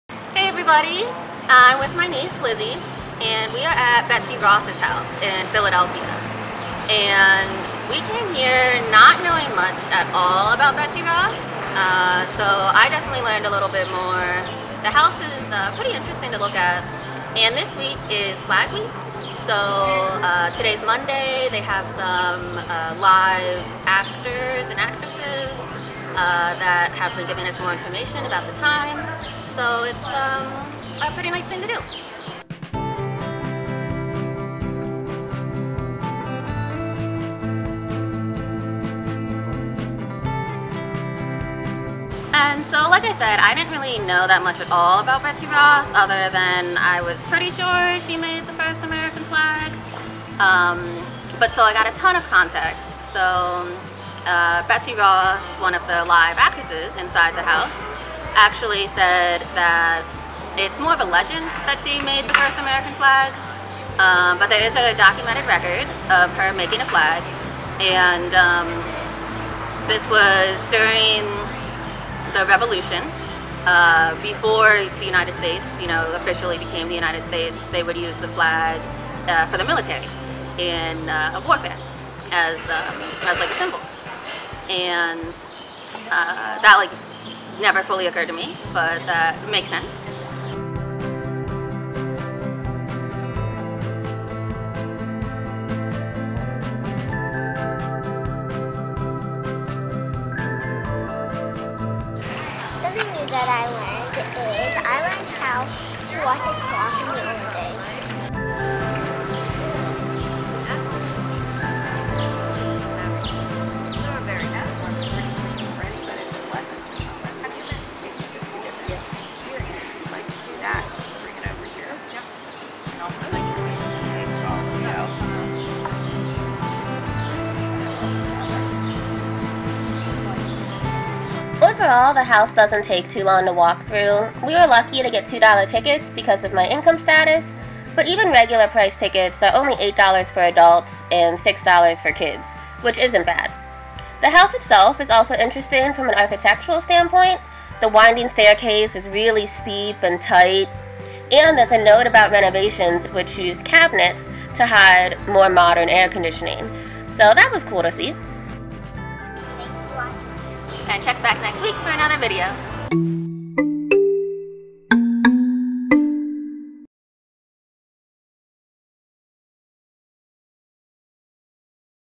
The Betsy Ross House on Flag Day - Philadelphia, PA - Review
Exploring the local area with my niece & still trying my best to social distance. This was a nice weekend.